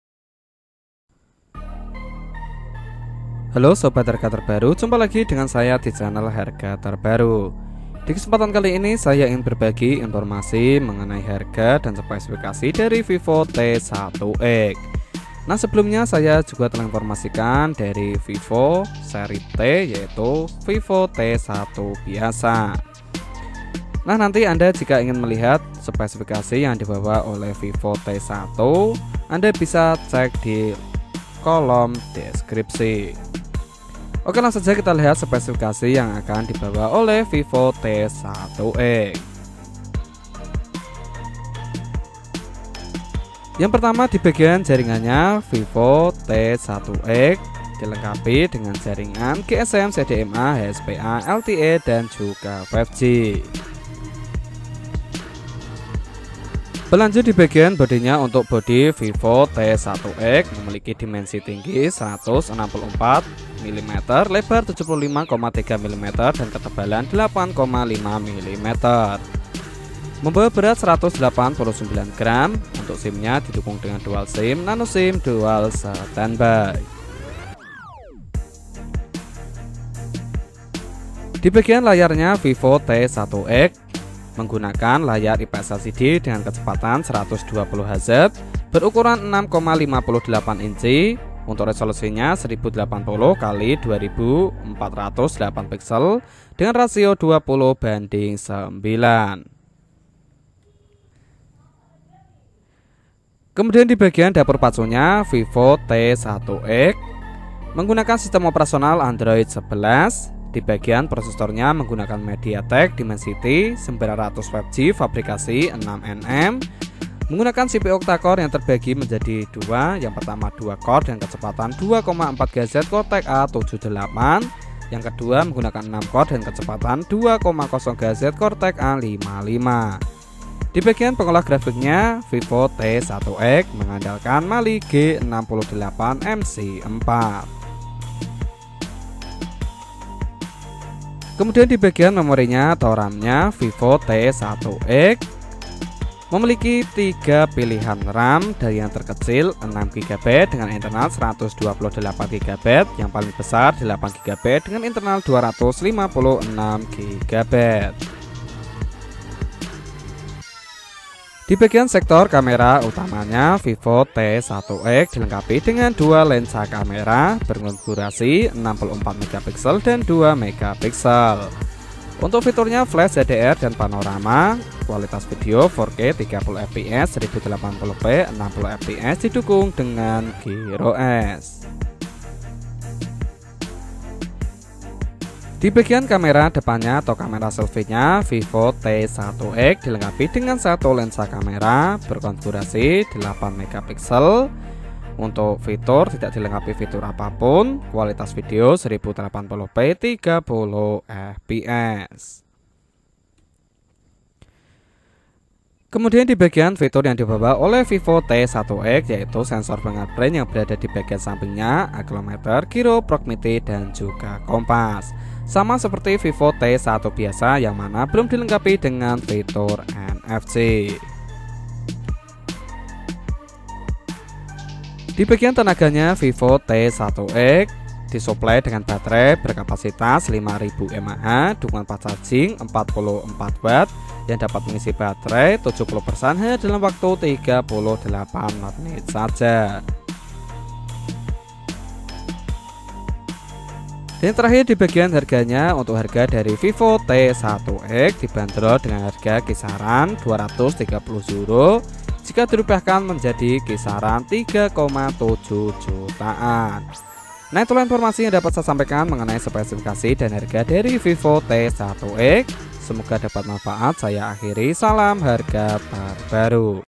HARGA TERBARU HP VIVO T1X SPESIFIKASI LENGKAP
Vivo T1x mengusung layar IPS LCD yang mendukung refresh rate120Hz. Namun untuk Vivo T1X ukurannya 6,58 inci dengan resolusi 1.080 x 2408 piksel.

Vivo T1X menggunakan prosesor MediaTek Dimensity 900 5G (6nm) yang dipadu dengan tiga opsi memori, yakni 6 GB/128GB, 8GB/128GB, dan 8GB/256GB.